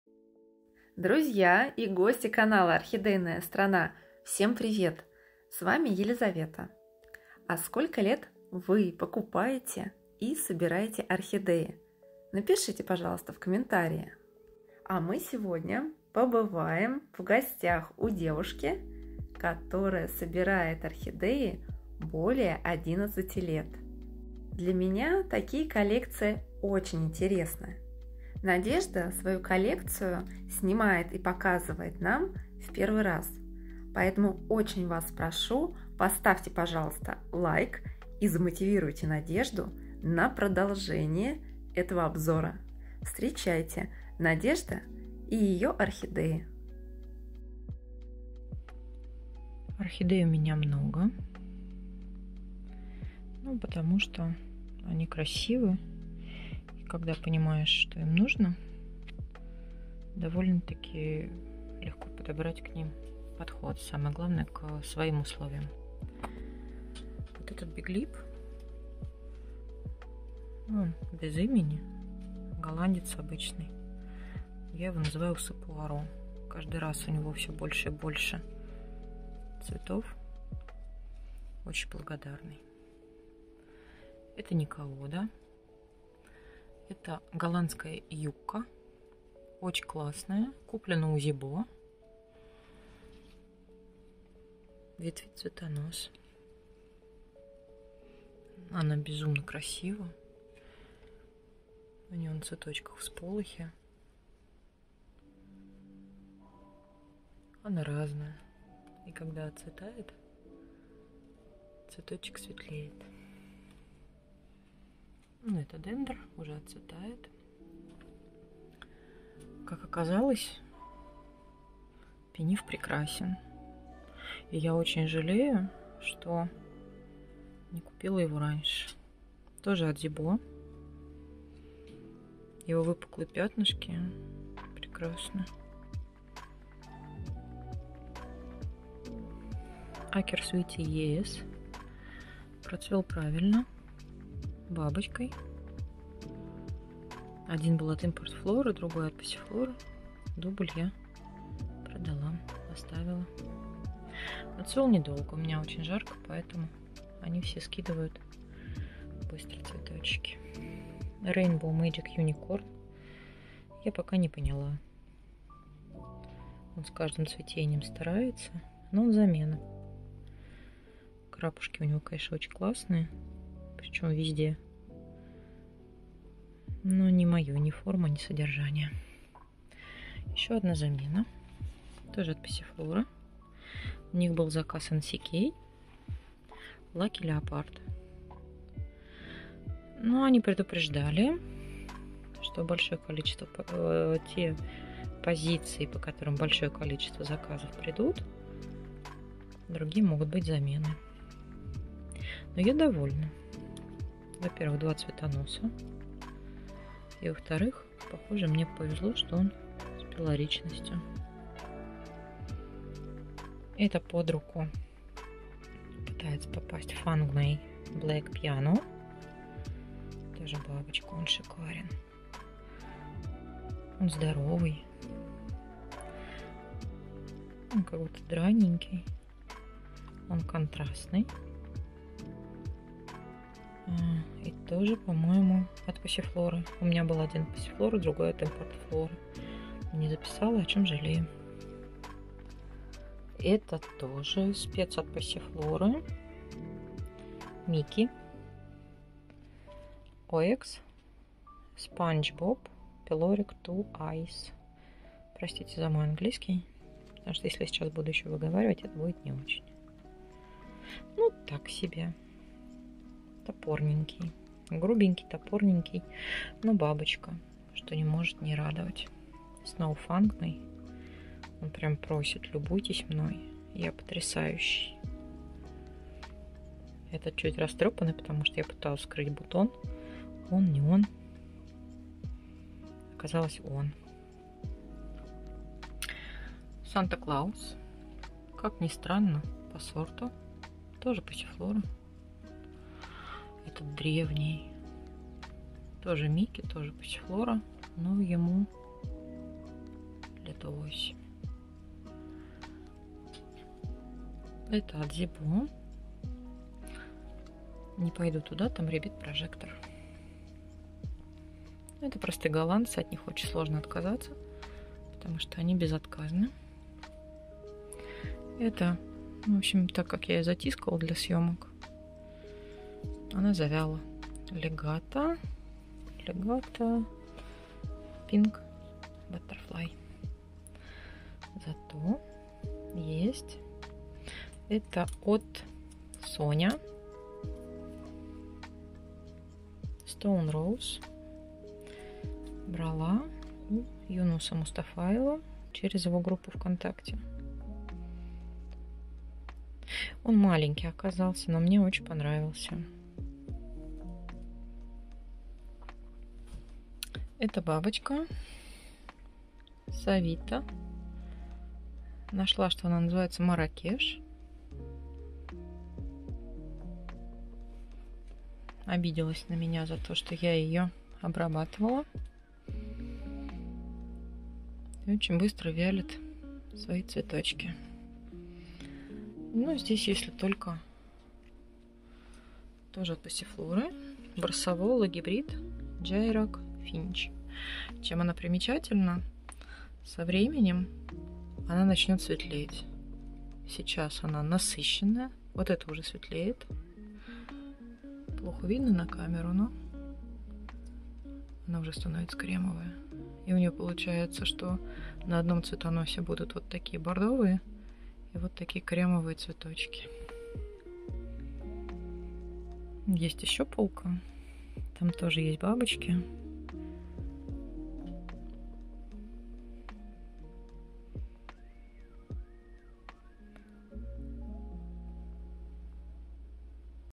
В гостях у Надежды! Коллекция орхидей которой 11 лет, в которой живут орхидеи разных видов.
Для Надежды - это первое видео небольшой части своих орхидей. Она человек, собиратель орхидей с тонким вкусом и большим опытом.

Тайм коды:

01:10 P. Poirot Орхидея Усы Пуаро или P. Reyoung Prince (Биг Лип Рейонг Принц или Рейянг Принц)
01:27 P. Pretty Yuka орхидея Юка или Юкка от Зебо
01:54 Дендробиум Dendrobium
02:00 P. Pinif Пиниф от Зебо

02:20 P. Acker Sweetie ES (Акер Свит ес)
Dtps. Acker's Sweetie 'Dragon Tree Maple' SM/TOGA
Phal. Anmio Gentleman ‘AN-6608’ (Анмио Гентельман)

02:40 P. Allura Tropica или P. Golden Magic Unicorn - Биг лип - замена от Пассифлоры

03:00 P. Lucky Leopard (орхидея Лаки Леопард) от NCK бабочка пелорик, цветёт на двух цветоносах.

03:34 P. Fangmei Black Piano 'FM-1552’ Фаленоасис Фангмей Блэк Пиано. Крупные яркие цветки бабочки, верхняя и нижние лепестки белые, с бордовой окантовкой и крапом по центру, а боковые петалии розовые также с крапом по центру.

04:03 P. Sponge Bob (peloric - 2 eyes) от поставщика Мики орхид

04:46 P. Santa Claus или орхидея Аллюра Санта Клаус

04:55 от Мики а-ля Мейджик АРТ точное название неизвестно - замена от Пассифлоры

05:05 P. Saparita орхидея Сапорита от Зебо

05:14
05:26 P. Legato pink butterfly peloric

05:35 SON-YA MC-RT1 Stone Rose Каменная роза Stone rose, поставщик Son Ya

05:58 P. Marrakesh peloric орхидея бабочка Марракеш или

06:24 Brassocatanthe Jairak Finch,  hybrid гибрид Брассаволы, Каттлея

Как поливать орхидеи.
Полив.
Уход за орхидеями.
Пересадка фаленопсиса.